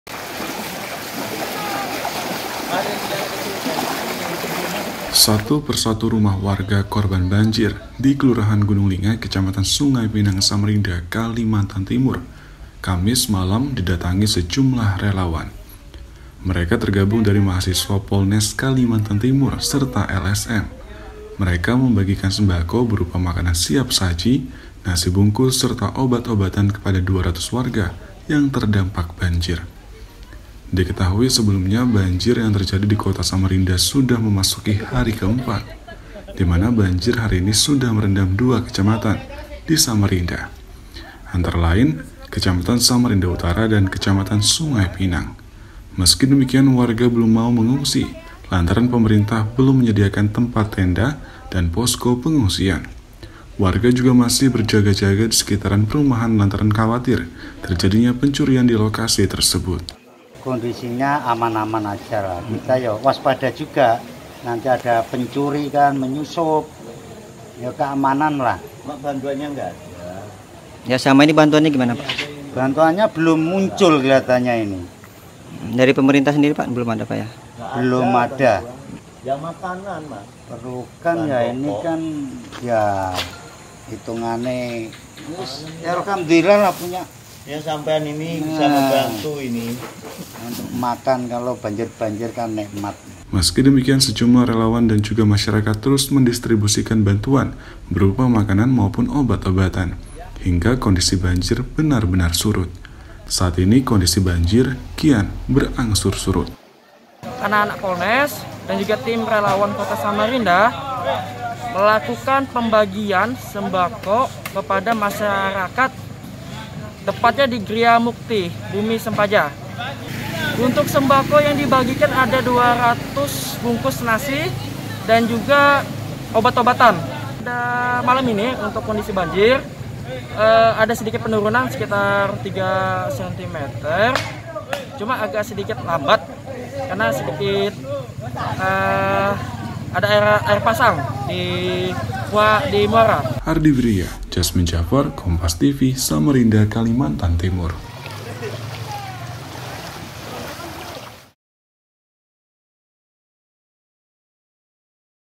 Relawan Bagikan Nasi Bungkus Untuk Korban Banjir
SAMARINDA, KOMPAS.TV  Satu-persatu rumah warga korban banjir di Kelurahan Gunung Lingai, Kecamatan Sungai Pinang, Samarinda, Kalimantan Timur, kamis malam didatangi sejumlah relawan.

Mereka tergabung dari mahasiswa polnes Kalimatan Timur serta LSM, mereka membagikan sembako berupa makanan siap saji, nasi bungkus serta obat-obatan kepada 200 warga yang terdampak banjir.

Diketahui sebelumnya, banjir yang terjadi di Kota Samarinda sudah memasuki hari ke-4, dimana banjir hari ini sudah merendam 2 kecamatan di Samarinda. Antara lain, Kecamatan Samarinda Utara dan Kecamatan Sungai Pinang.

Meski demikian warga belum mau mengungsi, lantaran pemerintah belum menyediakan tempat tenda dan posko pengungsian. Warga juga masih berjaga-jaga di sekitaran perumahan lantaran khawatir terjadinya pencurian di lokasi tersebut.

Meski demikian, sejumlah relawan dan juga masyarakat  terus mendistribusikan bantuan berupa makan maupun obat-obatan, hingga kondisi banjir benar-benar surut, saat ini kondisi banjir kian berangsur surut.